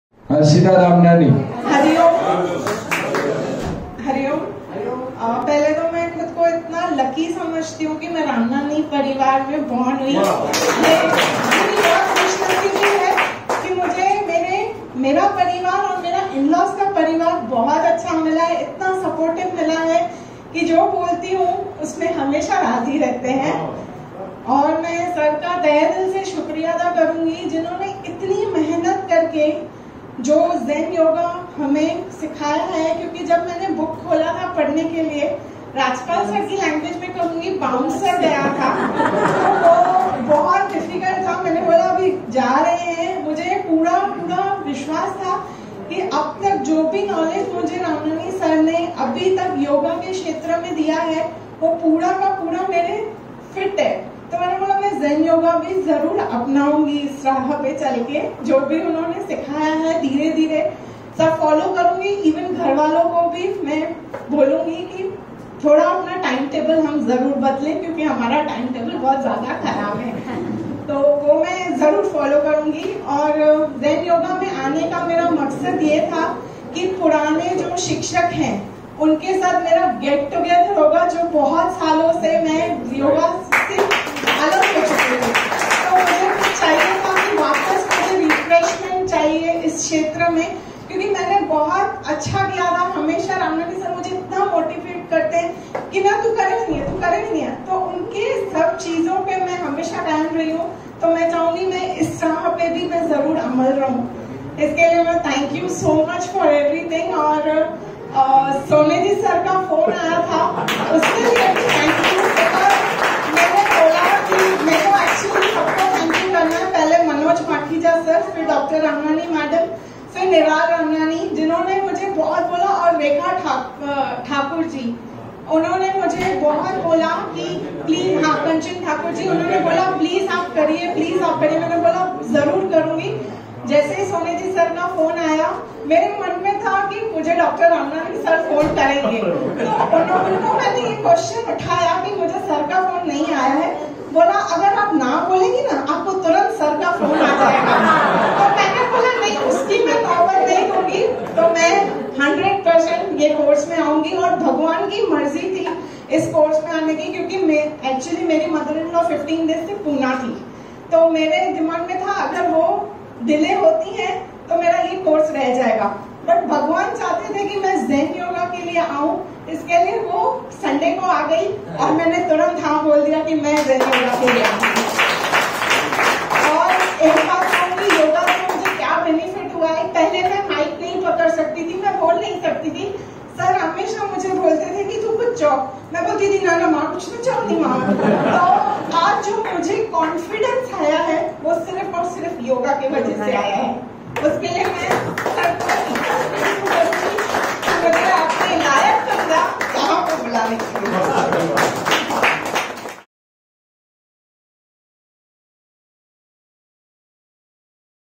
Zen Yoga | How I Built My Confidence | Harshita Ramnani Navani
In this video,  Harshita Ramnani Navani sharing her Experiences about the Zenyoga Prathmic Crash Course what she had learned, practiced, enjoyed in that course & in those 2 days (23rd July - 25th July 2021) at Dr. Saherji's International Zenyoga IESM Foundation, Badlapur.

Zen yoga: A Creative Psychotherapy to Self-integration